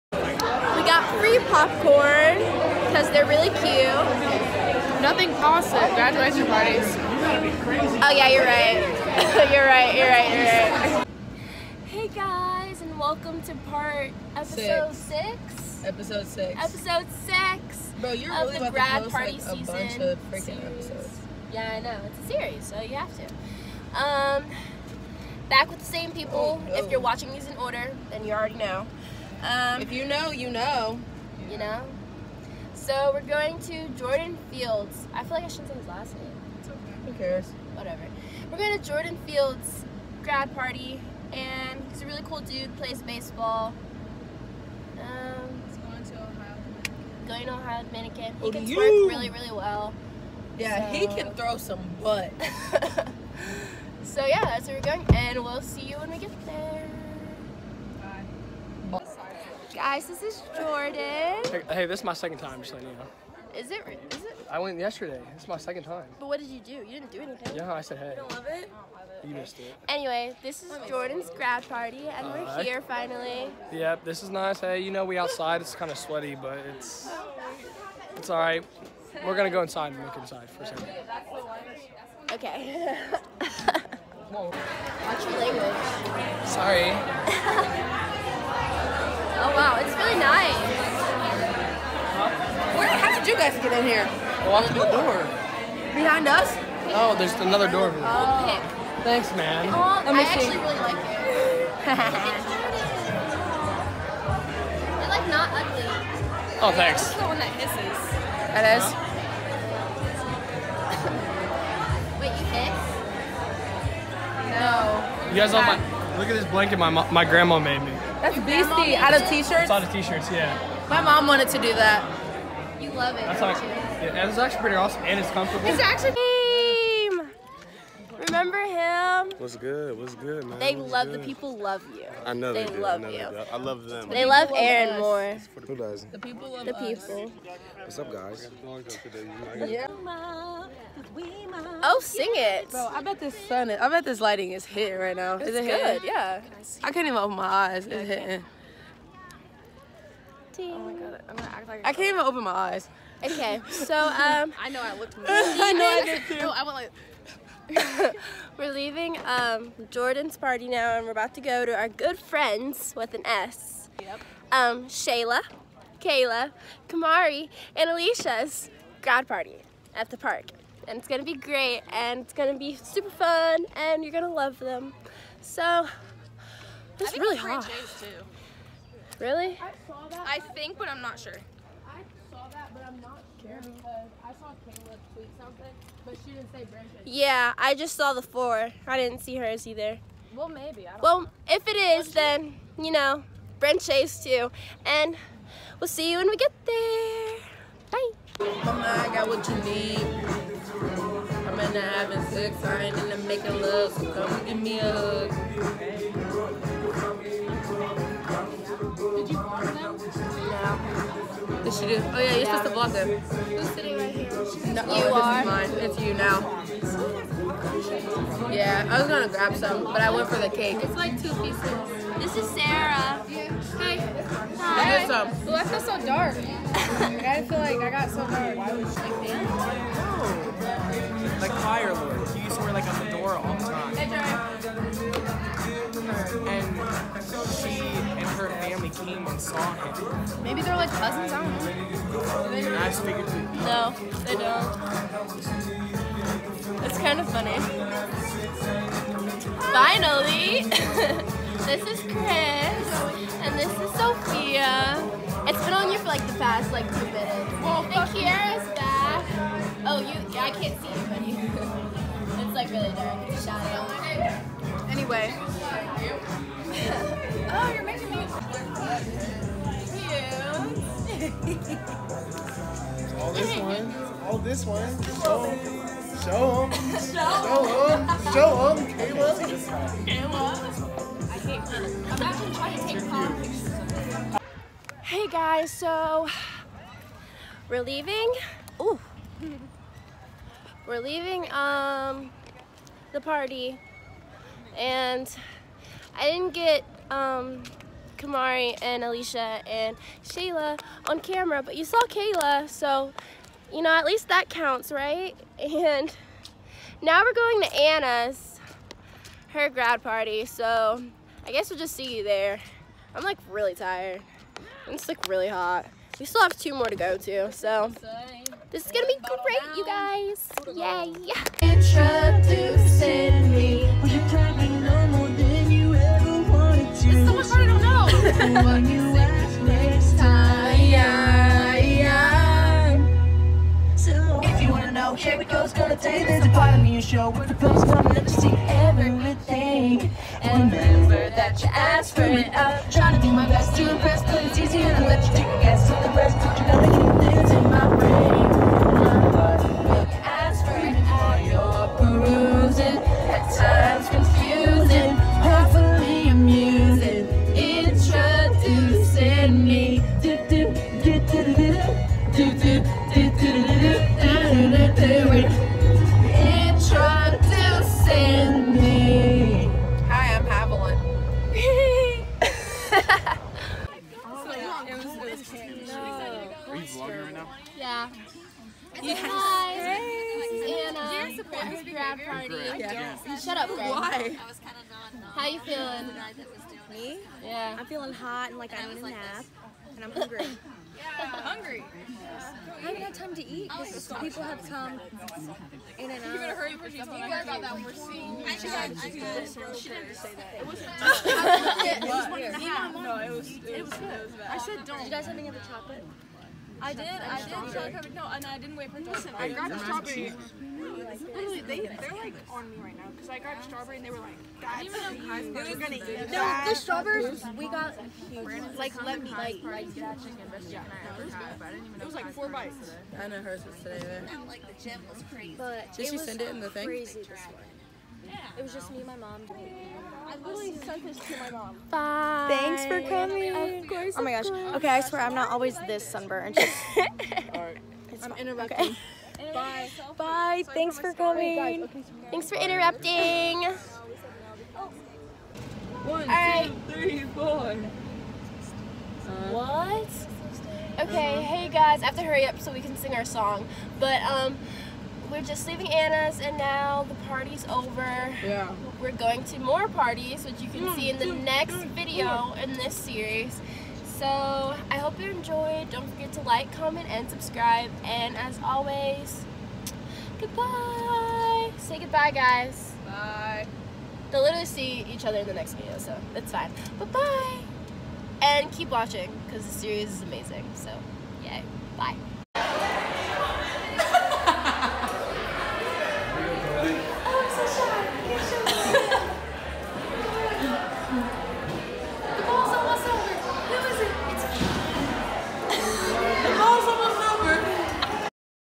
Grad Party Szn: Jordan, Shayla, Kayla, Alycia, Kamari and Anna | Grad Party Vlog
Hey guys and welcome the newest addition to the grad party szn series!

In this video we went to three grad parties!! It was super fun and I know everyone will is gonna do great things in the efuture!